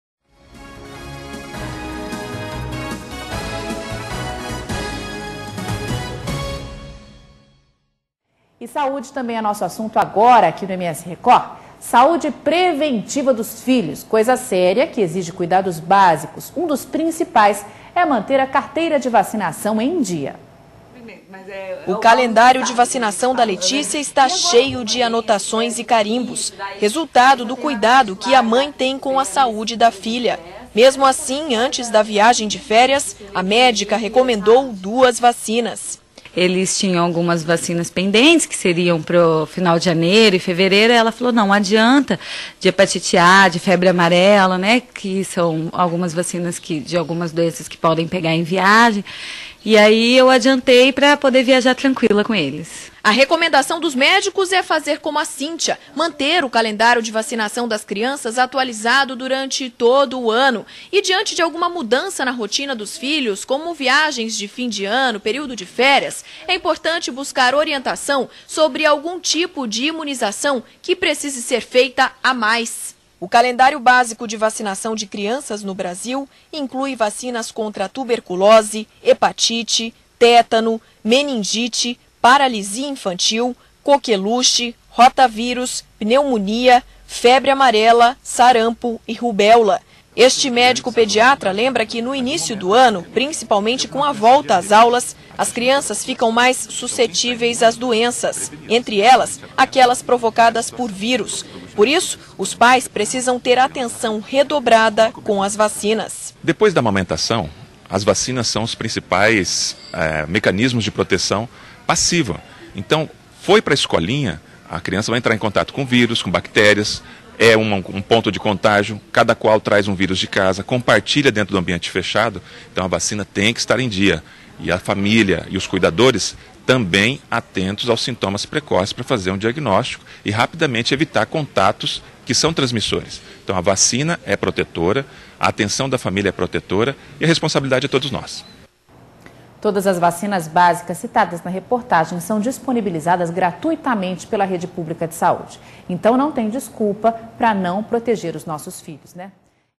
MS Record 1ª Edição - Pais devem ficar atentos com vacinações dos filhos
Saúde preventiva dos filhos, coisa séria que exige cuidados básicos. Um dos principais é manter a carteira de vacinação em dia.